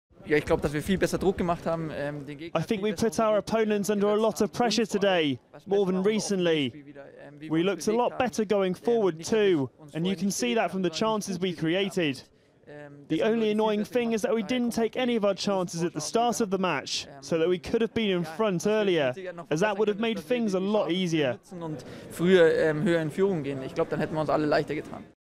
Lahm pleased with improving Bayern
Bayern Munich captain Philipp Lahm was pleased with the improvements across the pitch from his side as they beat Schalke 2-0 in the Bundesliga on Sunday.